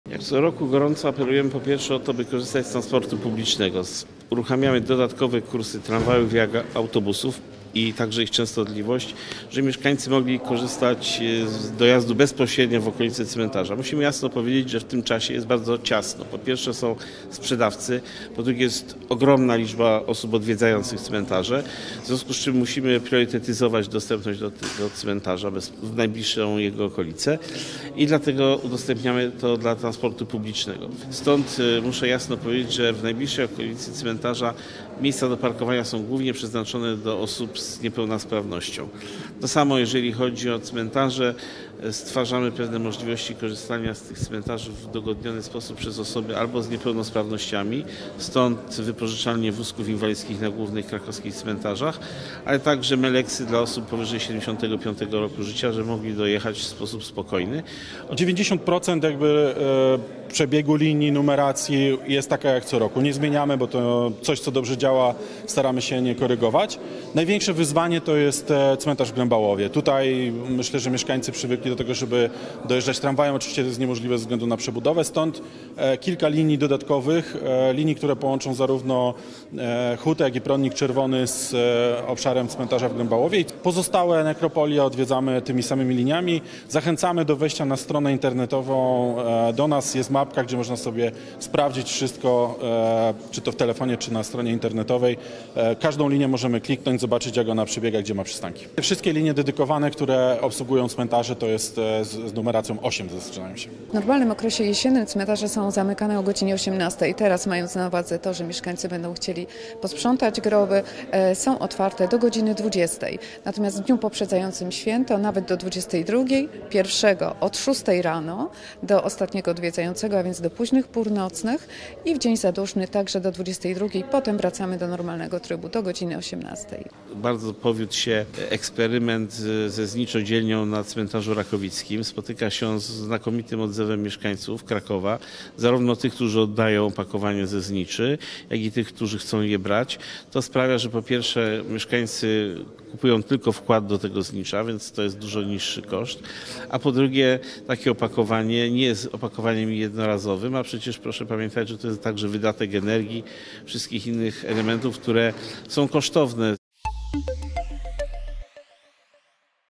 Wszystkich Świętych – informacje praktyczne
Zmiany w komunikacji miejskiej i organizacji ruchu drogowego w obrębie krakowskich cmentarzy, informacje praktyczne dotyczące infrastruktury cmentarnej oraz pomocy dla osób z niepełnosprawnościami.